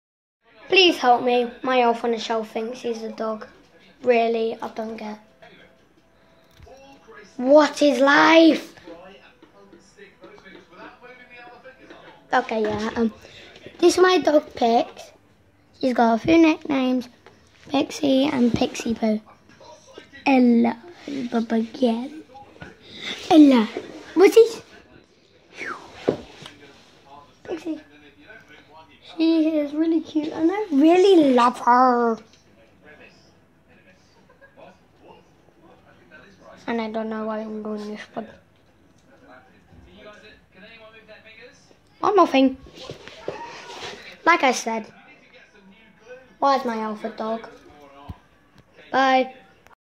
My elf on the shelf thinks that he's a dog
I have no idea why 🐶🐶